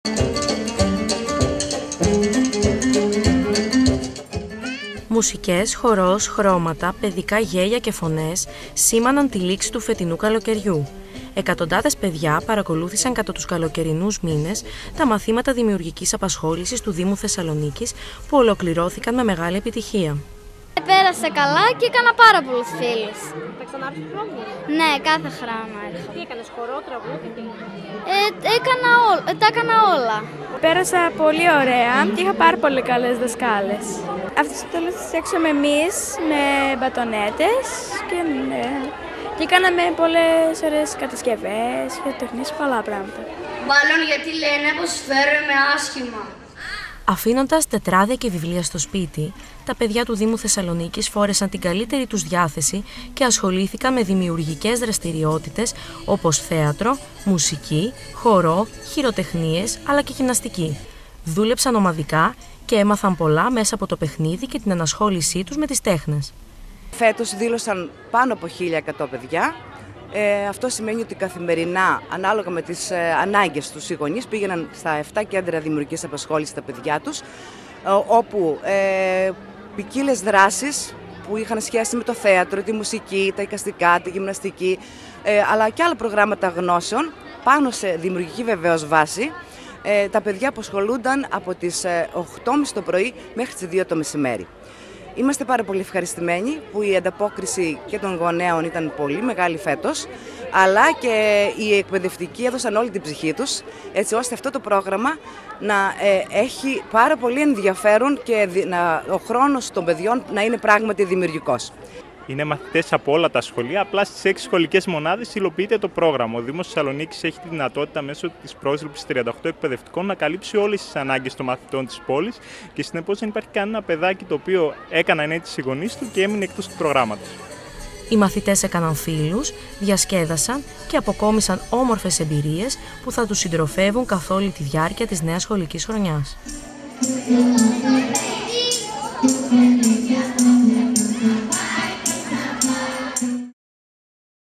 ΛΗΞΗ ΠΡΟΓΡΑΜΜΑΤΩΝ ΔΗΜΙΟΥΡΓΙΚΗΣ ΑΠΑΣΧΟΛΗΣΗΣ Δ.Θ. (TV100-010916)
Σε εορταστικό κλίμα ολοκληρώθηκαν τα μαθήματα δημιουργικής απασχόλησης μαθητών του δήμου Θεσσαλονίκης. Τα παιδιά συμμετείχαν σε μια πολύχρωμη παράσταση, κατά την οποία είχαν την ευκαιρία να παρουσιάσουν όλα όσα έμαθαν στη διάρκεια του καλοκαιριού.
Ρεπορτάζ: Ναταλία Πολυχρονίδου, Σταυρούλα Στάικου.
Κάμερα: Νίκος Κακολύρης.
Μοντάζ: Πάνος Τσακίρης.
Μιλούν: Άντη Φωτοπούλου (Προϊσταμένη  του Τμήματος Προγραμμάτων και Δια Βίου Μάθησης), Αναστάσιος Τελίδης (Εντεταλμένος Δημοτικός Σύμβουλος για Δράσεις Παιδείας)